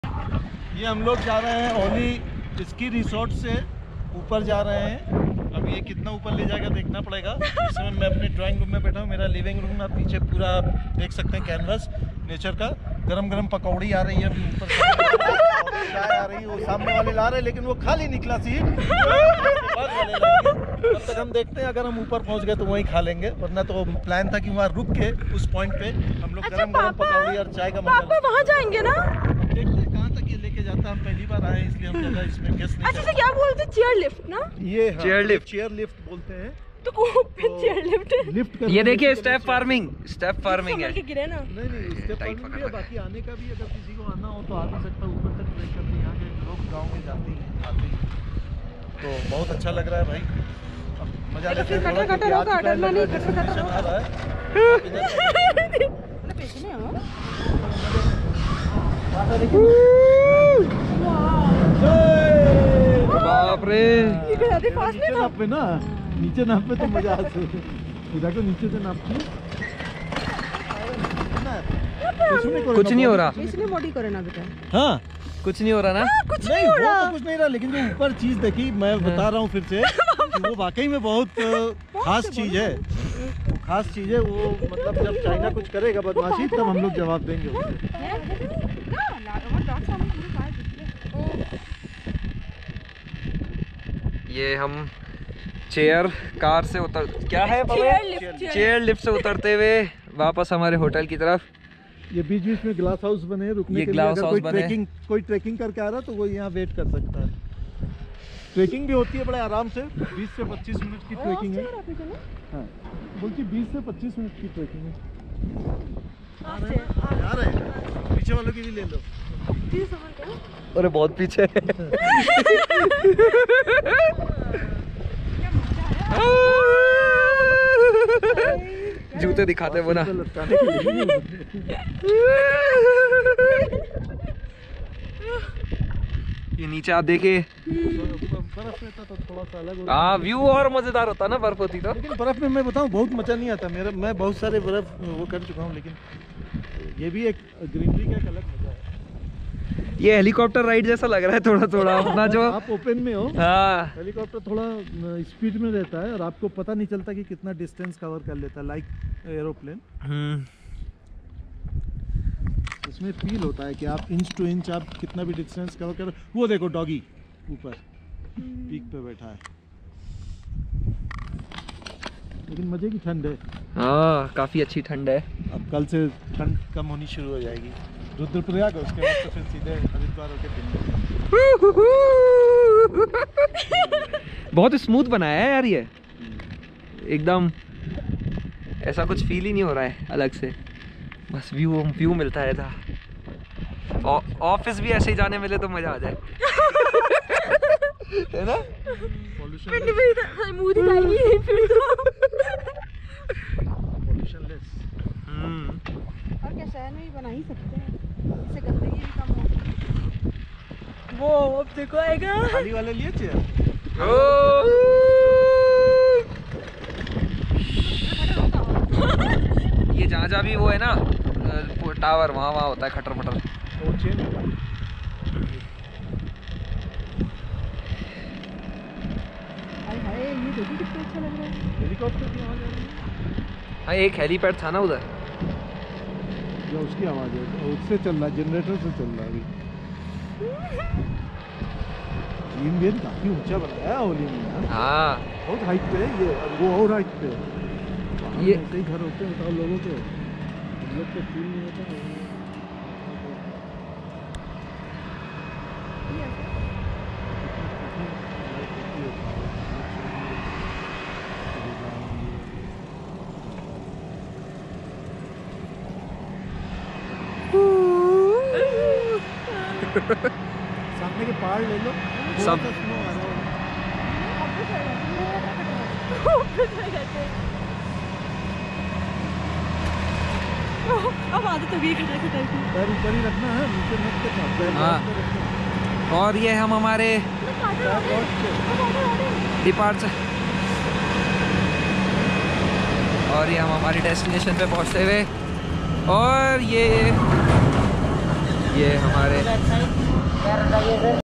Auli Chair Car Ride Fun with Family | Uttarakhand - October 2019 | Char Dham Yatra
Vlog of our chair car ride in Auli Uttrakhand | October 2019

Part of the Char Dham Yatra in October 2019